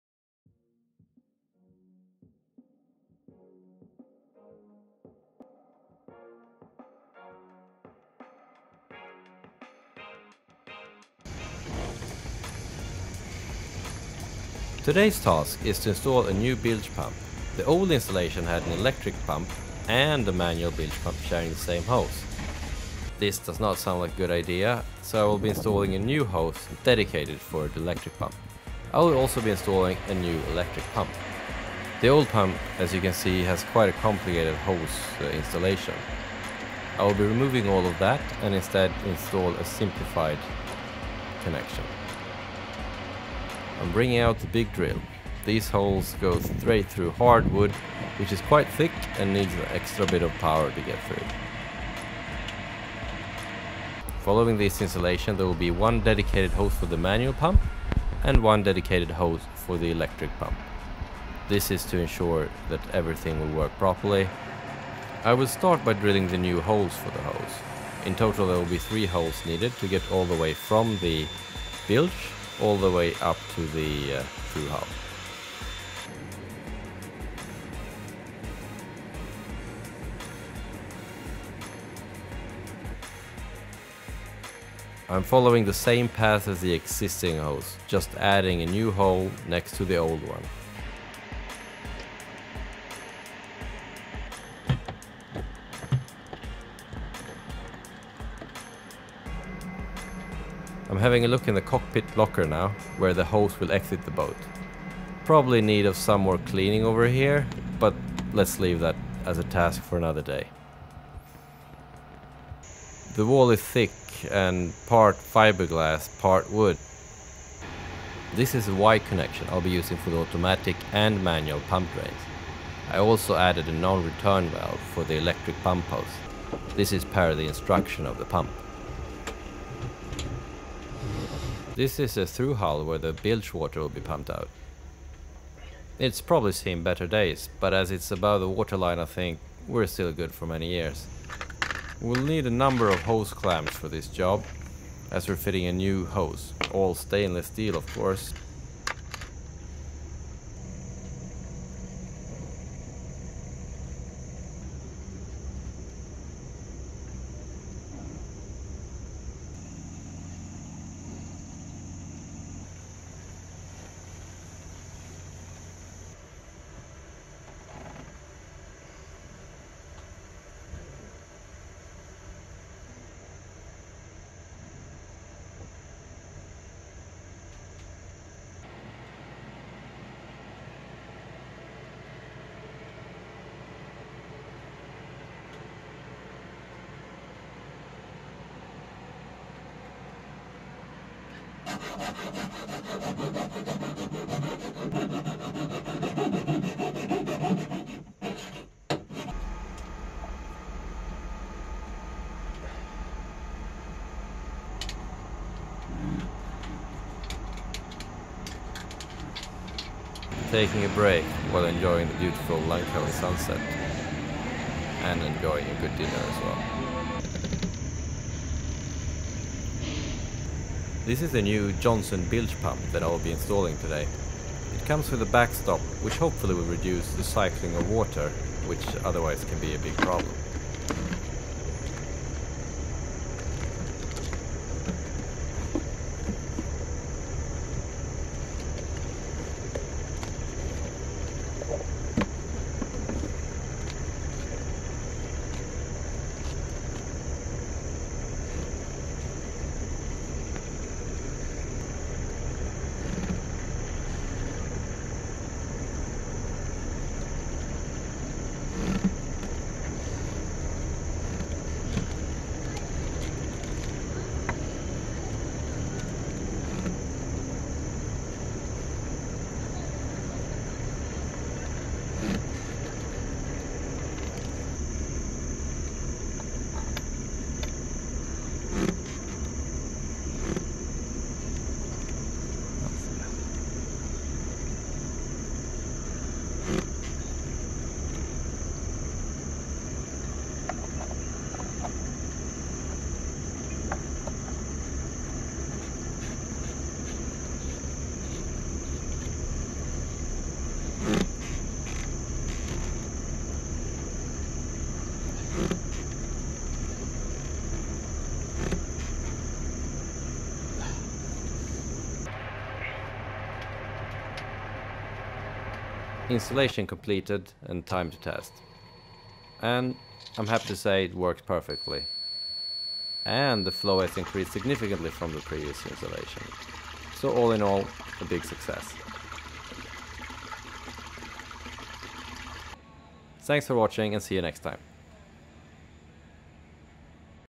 Sailing Scarlet - Bilge Pump Installation
Installing an electric bilge pump in a Najad 37 Sailing Yacht and unboxing of a Johnson 2200 GPH bilge pump from SPX flow. A bilge pump installation from start to finish.